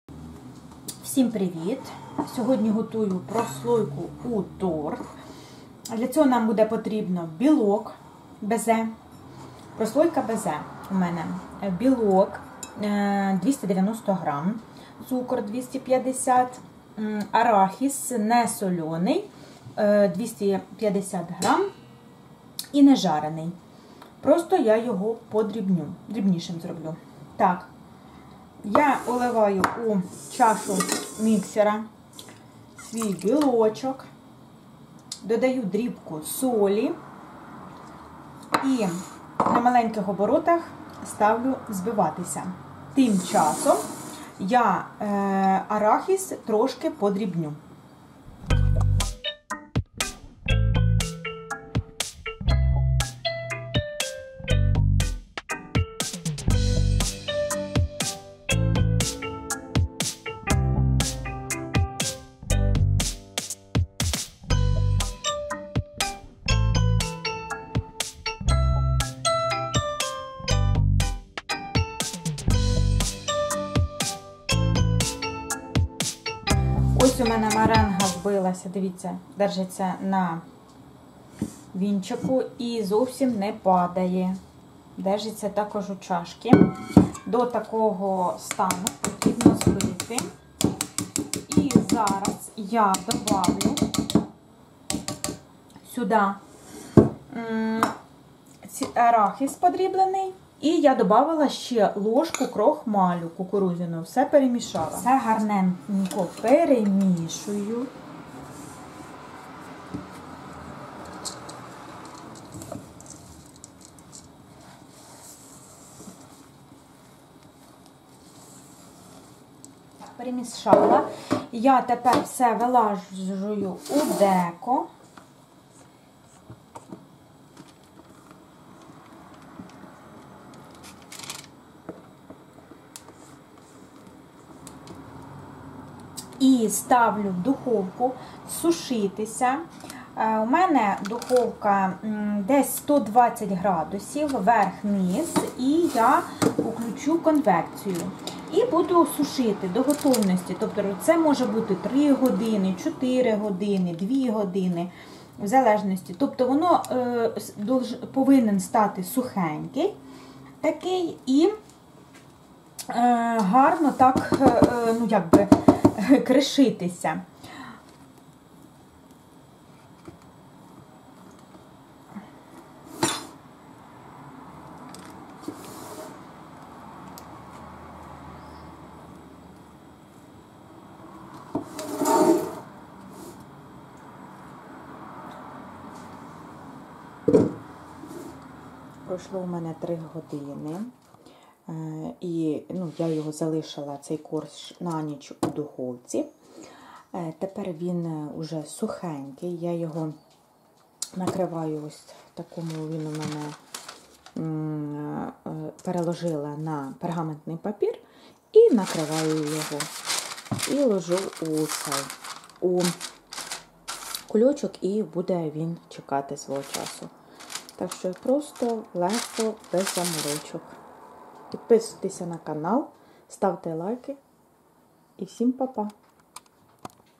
Прослойка у Київський торт!
Рецепт:
290г білка
250г цукру
250г арахісу
0,5ч.л. лимонної кислоти
1ст.л. кукурудзяний крохмаль